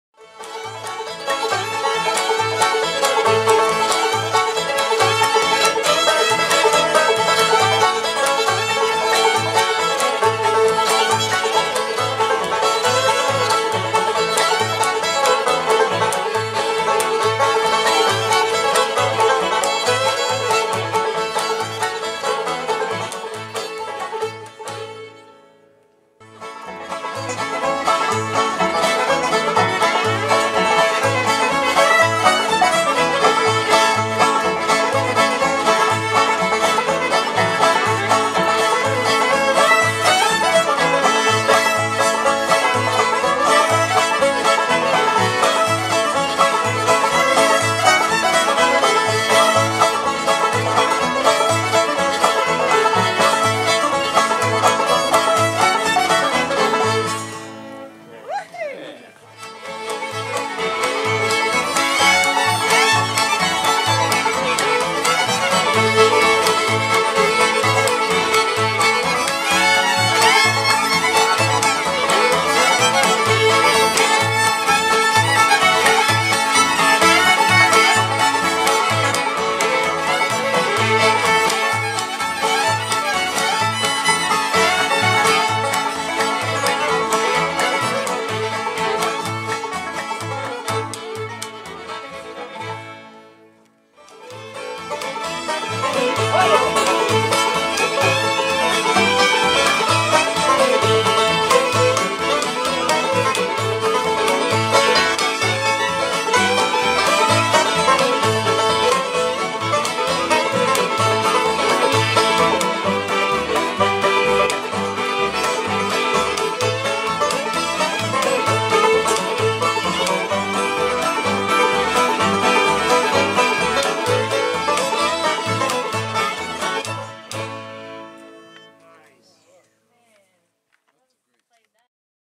Elk Creek Night Rambler's / Short clips 2022
Musician's from multi bands gather after ribbon's are handed out on Sat. night.